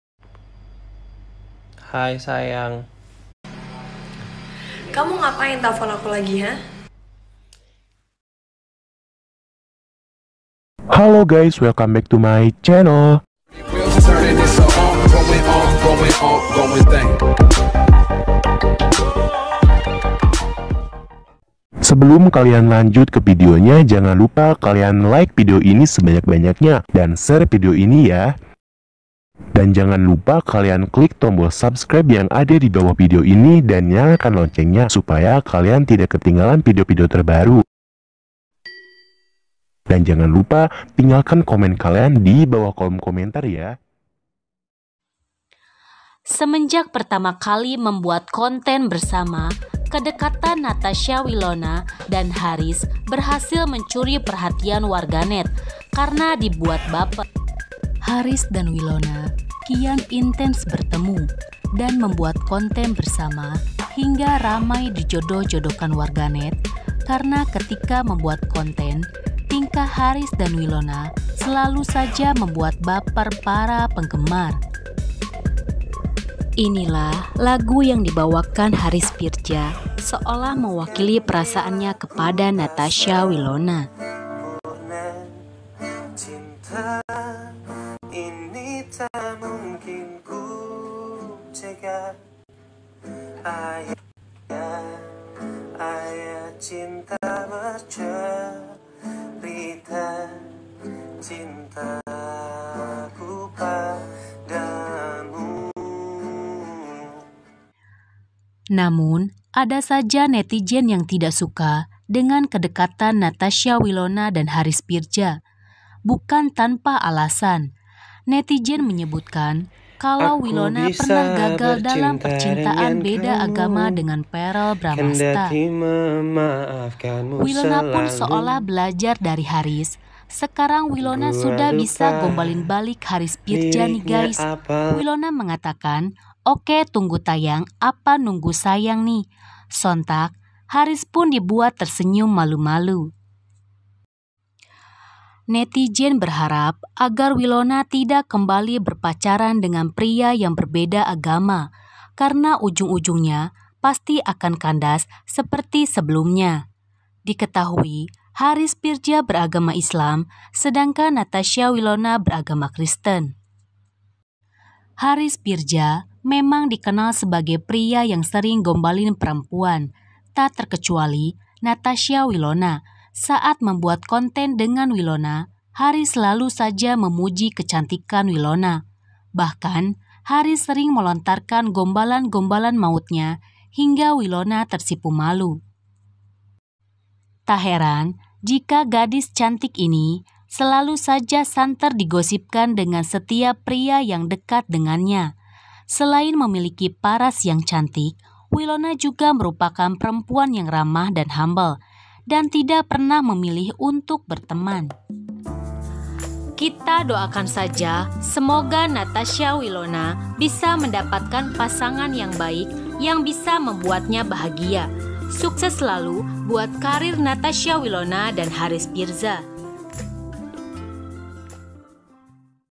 HARIS VIRZA MENYANYIKAN SEBUAH LAGU UNTUK NATASHA WILONA..!!
Kedekatan Wilona Dan Haris Virza,Yang Semakin santer terdengar setelah mereka sering intens beretemu membuat konten bersama..
Haris Virza pun seolah memberikan kode dengan menyanyikan sebuah lagu yang di tujukan kepada seorang wanita apakah Natasha Wilona..!!